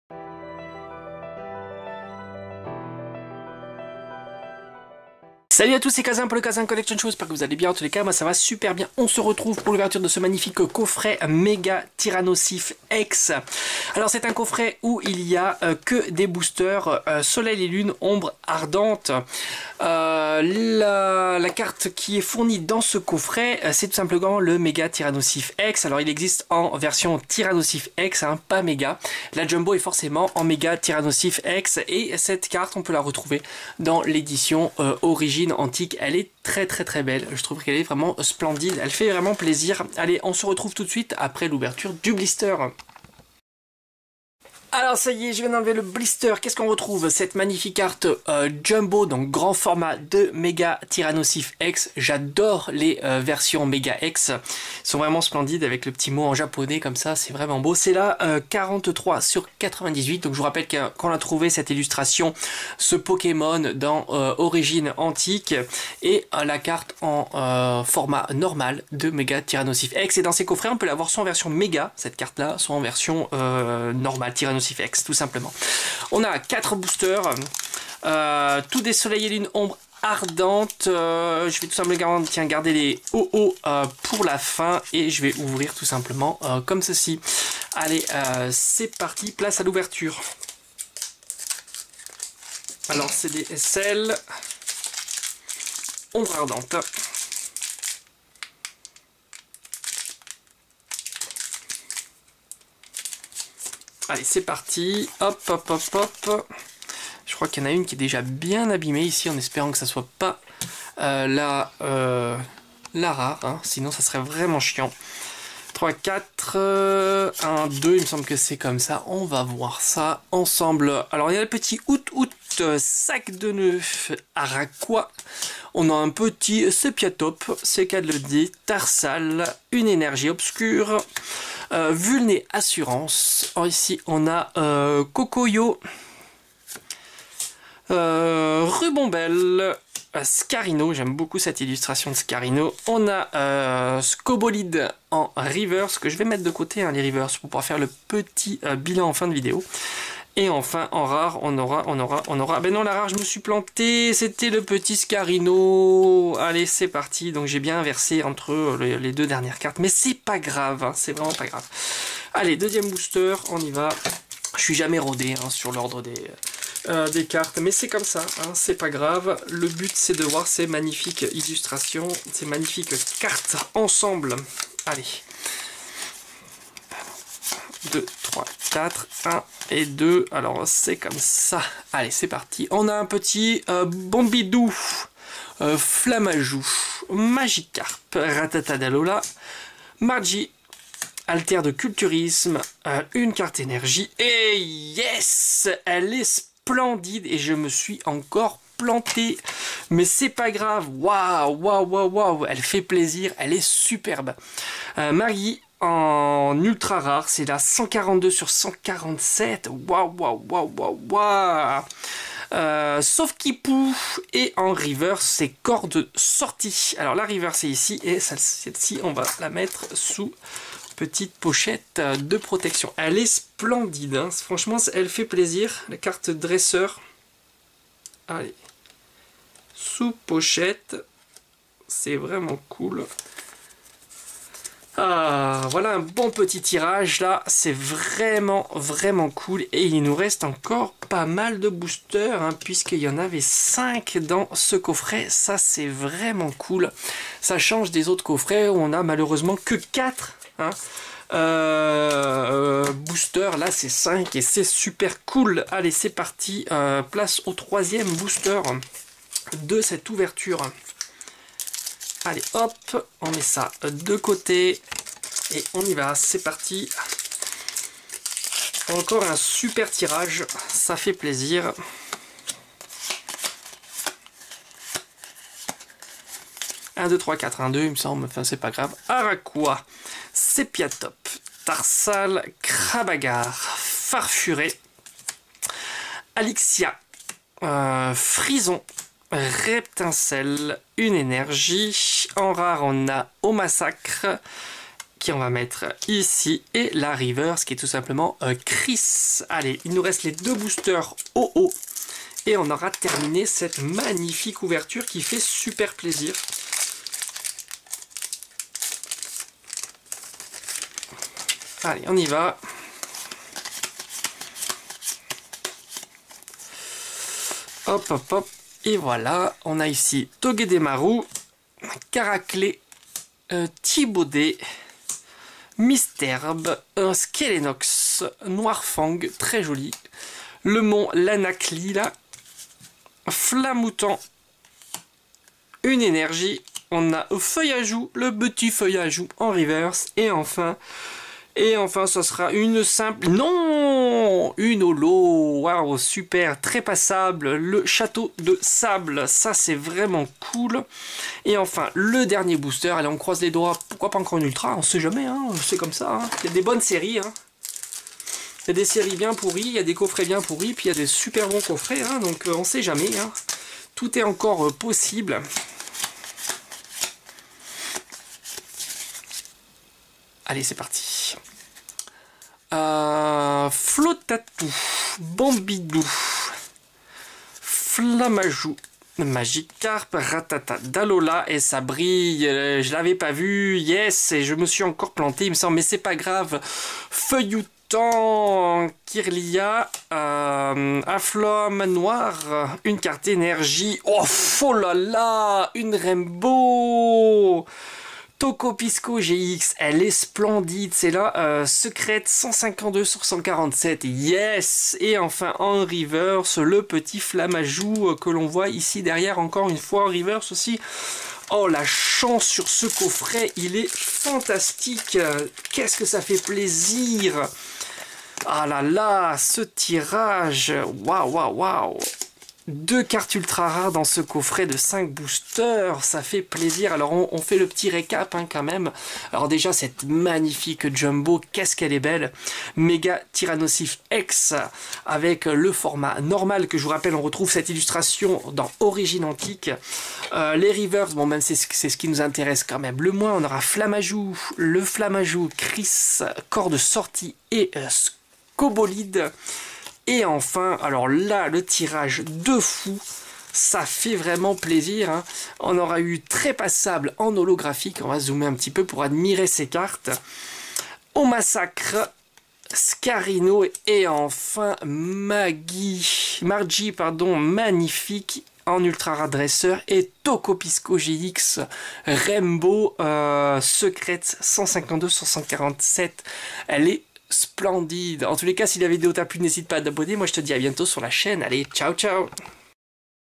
Ouverture du coffret Pokémon M Tyranocif EX une Rainbow! 😎
Ouverture du coffret Pokémon M Tyranocif EX!
5 boosters SL ombres ardentes.